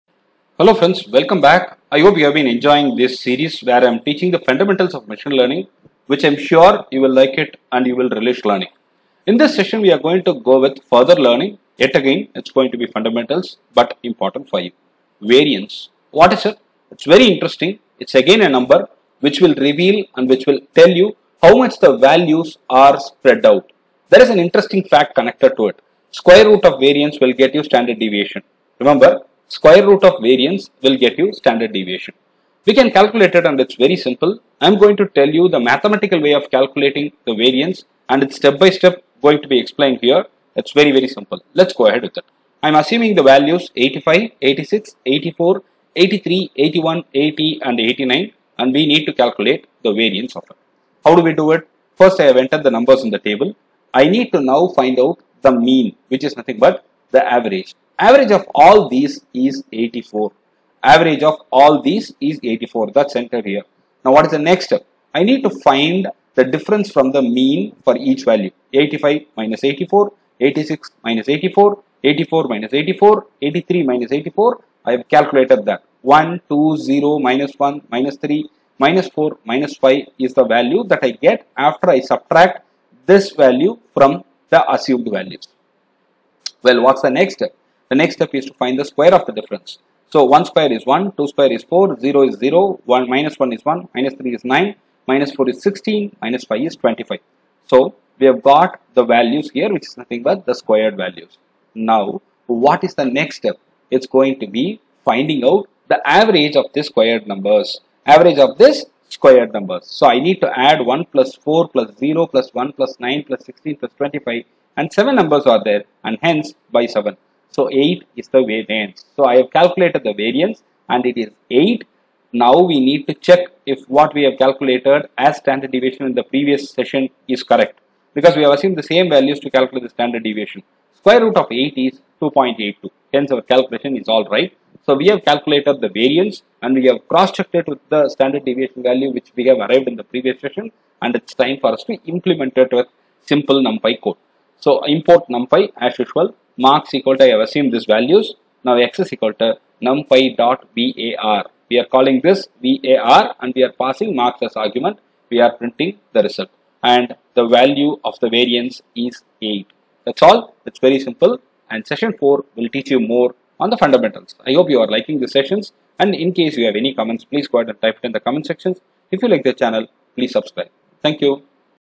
3. Machine Learning Fundamentals - What is Variance? How to calculate? How to implement?
Here you go, I explain the procedure to calculate variance and implement the same with python